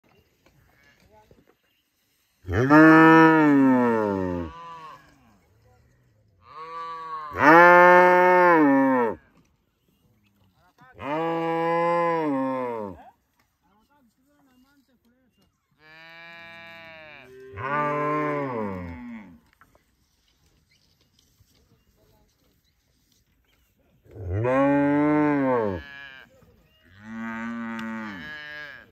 Cows of Africa - Beautiful cows in Butuku, Ntoroko District, Uganda
Beautiful herds of cattle in Butuku area that’s worth visiting